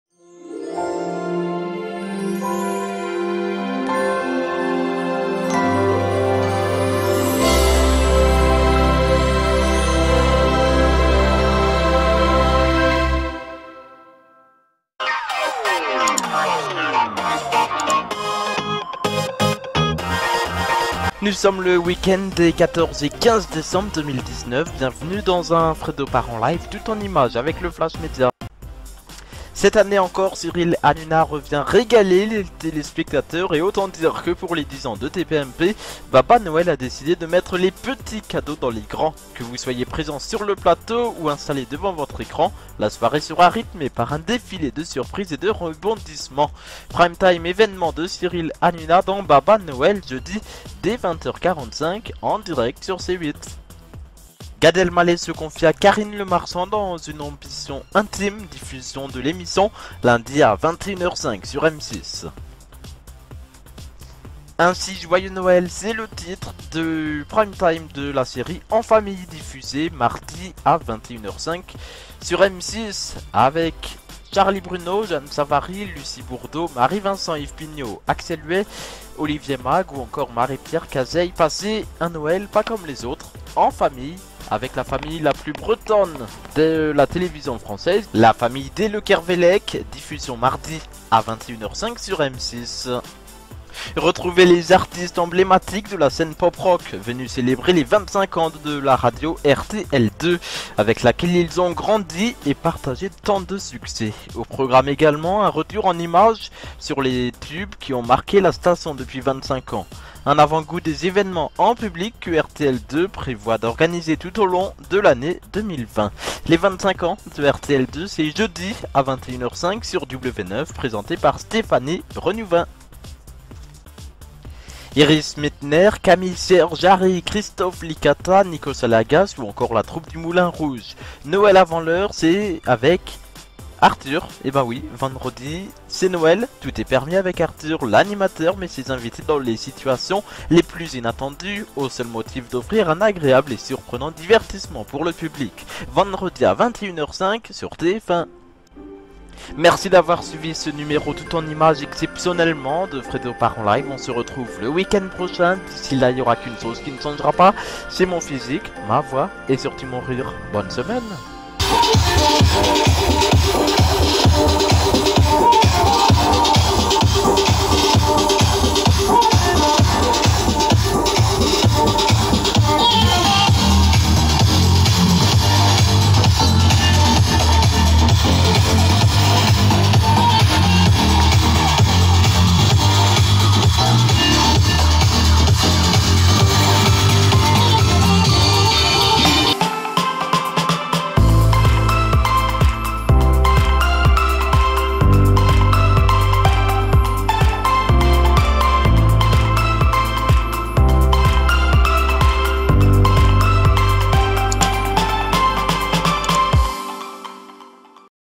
Tout est permis en famille (FredoPartEnLive)
Au sommaire de FredoPartEnLive
- Flash Média : Tout est permis avec Arthur, En Famille, Une Ambition Intime, TPMP Baba Noël

- Bonus : WarmUp d'Elliot sur FunRadio (54ème)
Réagissez FredoPartEnLive @fredopartenlive